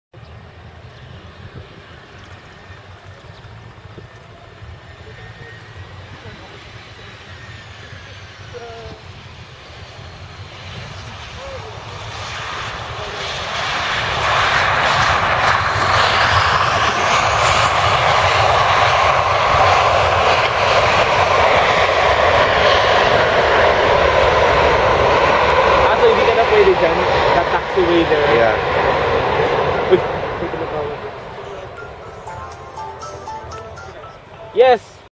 CEB - 5J Arrival RWY 04
Locations: Mactan - Cebu International Airport
Vantage Point : GenAv Area